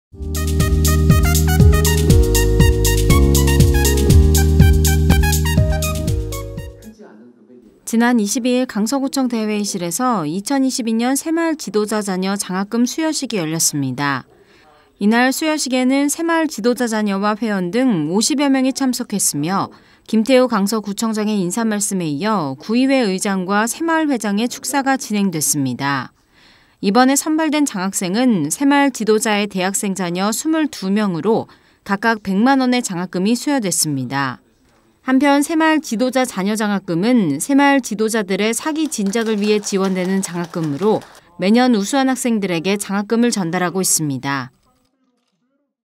2022년 새마을지도자 자녀 장학금 수여식
지난 22일 강서구청 대회의실에서
2022년 새마을지도자 자녀 장학금 수여식이 열렸습니다.

이날 수여식에는 새마을지도자 자녀와 회원 등 50여명이 참석했으며
김태우 강서구청장의 인사말씀에 이어
구의회 의장과 새마을회장의 축사가 진행됐습니다.

이번에 선발된 장학생은 새마을지도자의 대학생 자녀 22명으로
각각 100만 원의 장학금이 수여됐습니다.

한편 새마을지도자 자녀 장학금은
새마을지도자들의 사기 진작을 위해 지원되는 장학금으로
매년 우수한 학생들에게 장학금을 전달하고 있습니다.